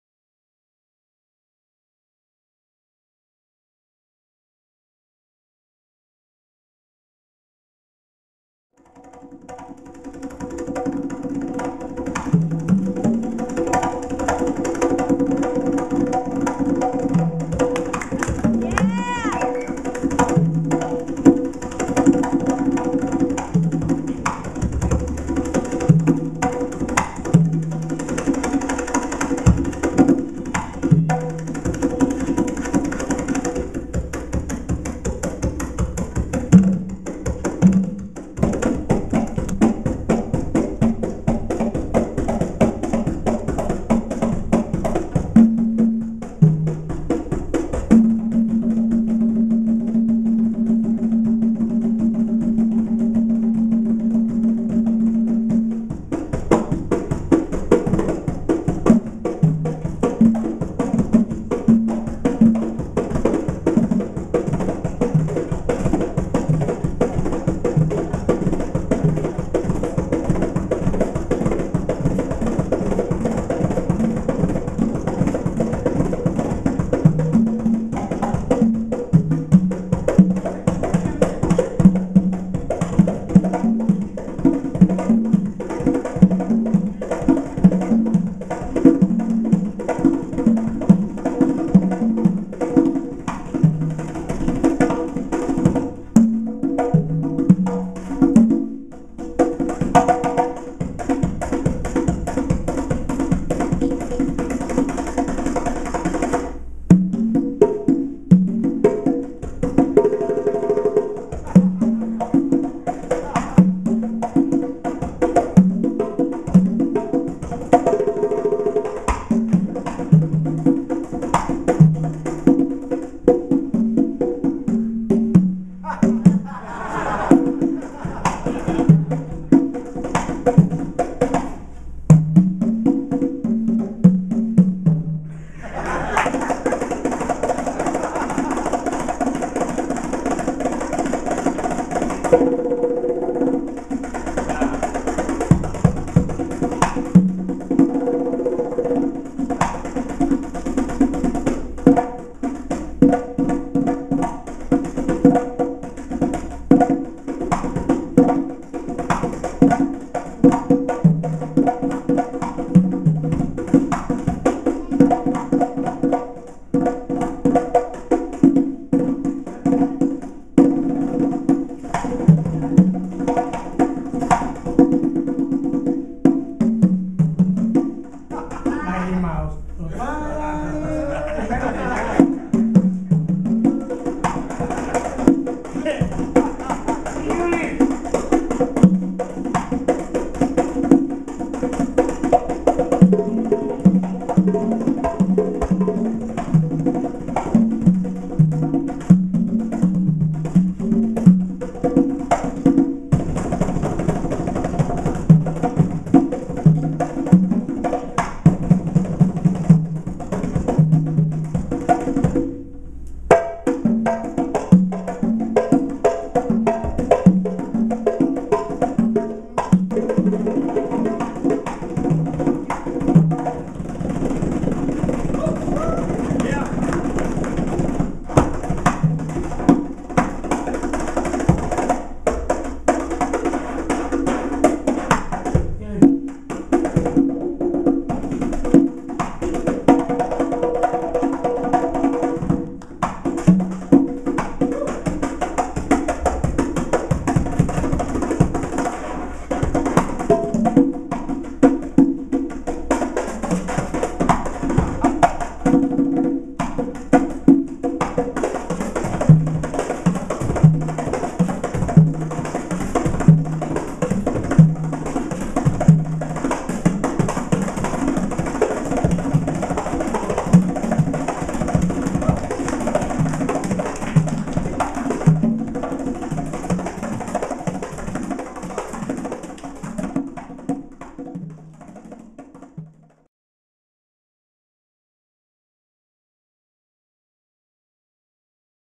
Hannah Ford/Giovanni Hidalgo Clinic Tour - Giovanni Solo Segment
Check out the master, Giovanni Hidalgo, gettin' it done at Memphis Drum Shop, the final stop of the Peace Love & Drums Clinic tour featuring Hannah Ford and Gio himself...

This man is amazing at his craft.